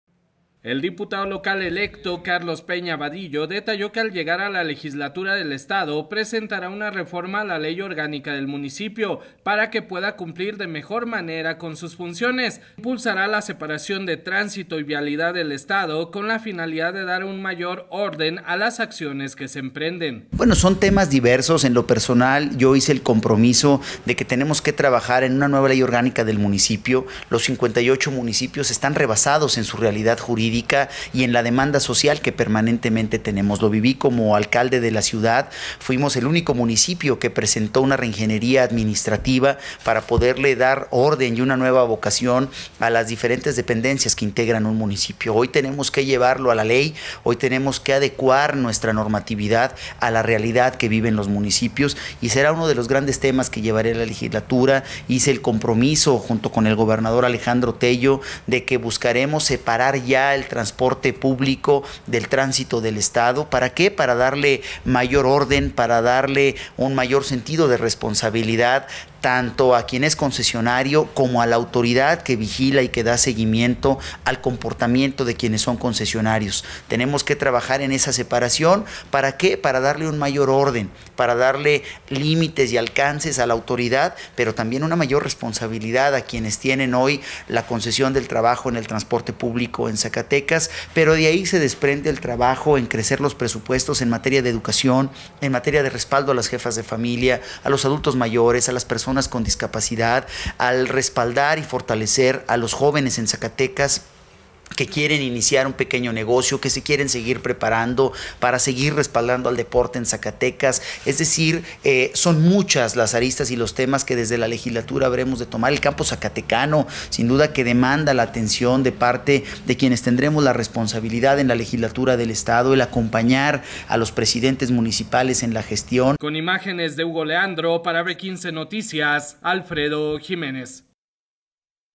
Carlos Peña Badillo, presentará reforma a la Ley orgánica del Municipio
El diputado local electo, Carlos Peña Badillo, detalló que al llegar a la Legislatura del Estado, presentará una reforma a la Ley Orgánica del Municipio, para que pueda cumplir de mejor manera con sus funciones, también impulsará la separación de tránsito y vialidad del estado, con la finalidad de dar un mayor orden a las acciones que se emprenden.